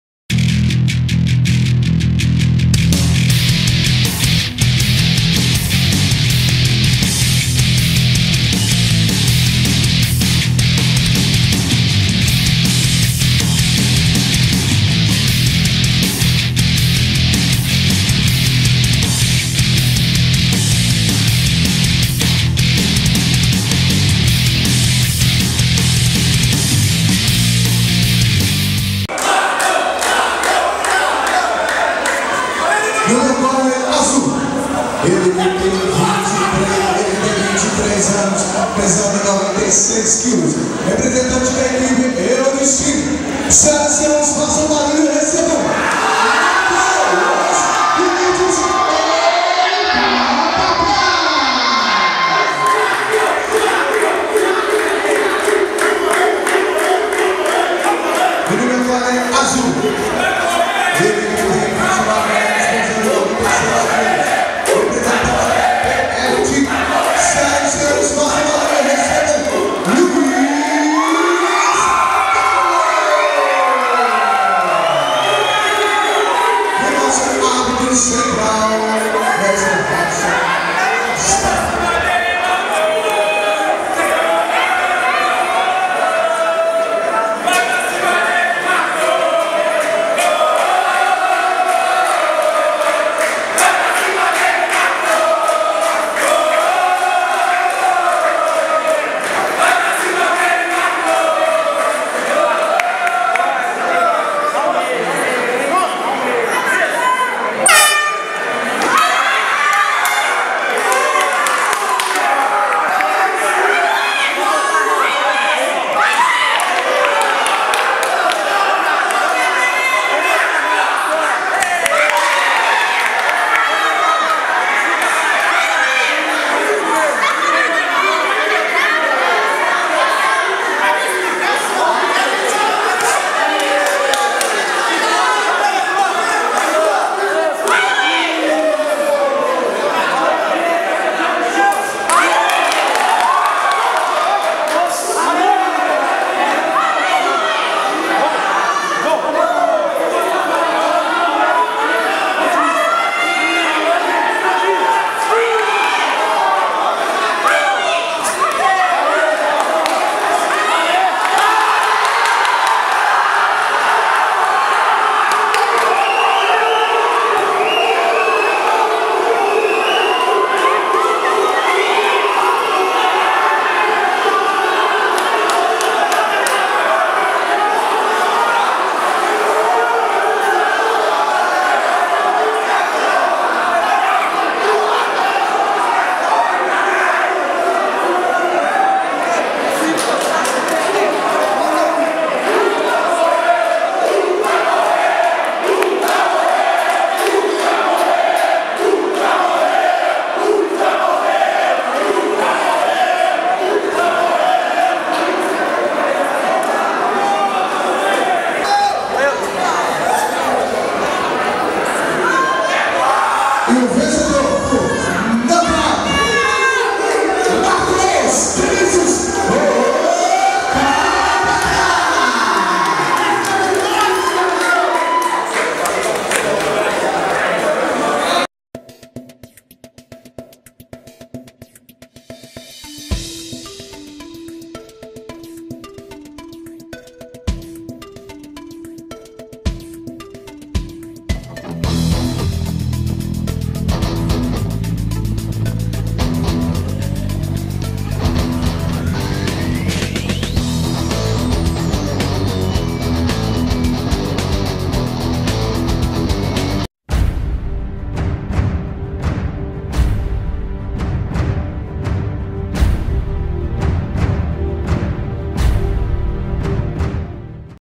Marcos vs Luiz - 2º Champions Fight TF Team
Em um dos combates de MMA amador do 2º Champions Fight TF Team, Marcos Vinicius (Eudes Team) venceu Luis Carlos (TF Team) com um belo nocaute no primeiro round do combate.